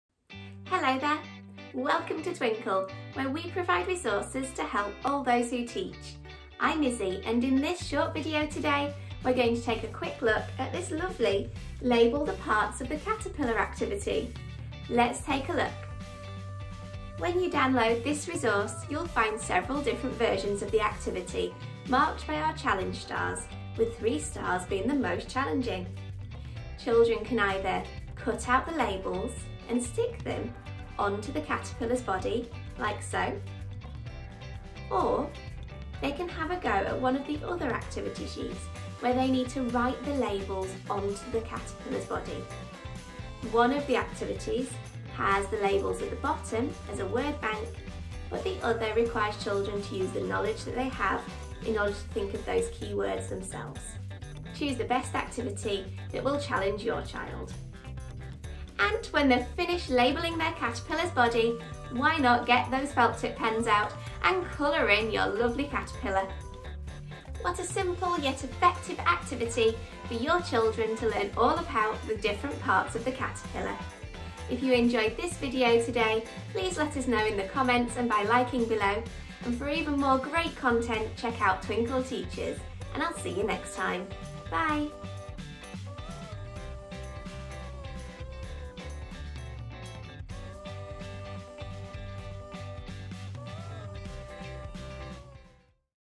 Label a Caterpillar
Use this Label the Parts of a Caterpillar Worksheet to aid your teaching ➡️

This resource is differentiated to challenge every child. The activities are marked with our star challenge rating.

In this video, Twinkl teacher Izzy will show you how to make the most of this resource and how to use each of the activities.

0:00 Intro
0:16 Download the resource
0:27 Cut and stick
0:40 Challenge